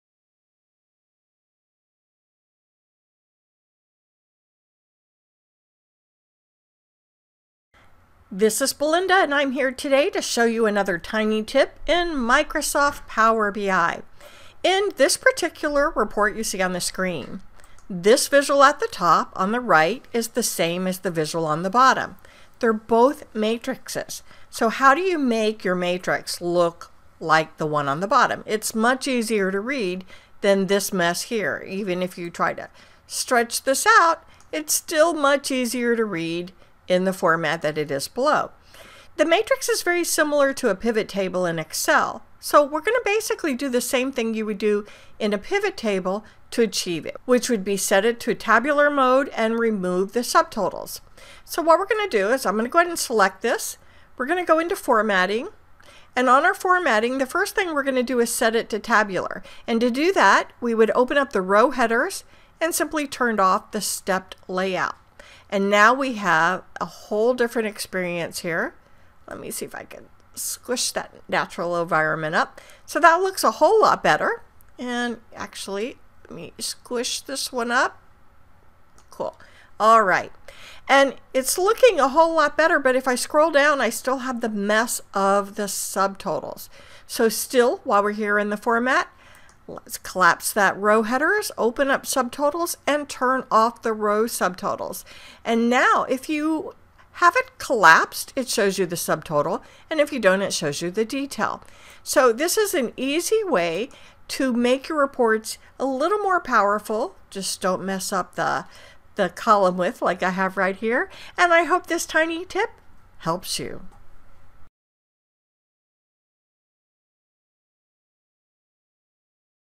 Formatting the Matrix Visual Microsoft Power BI
I love using PivotTables in Microsoft Excel.  When I use them, I always change them to Tabular and remove Subtotals to make them easier to read.  The Matrix visual in Microsoft Power BI is like the Excel PivotTable.  In this Tiny Tip video, I will show you how to make it tabular and remove subtotals.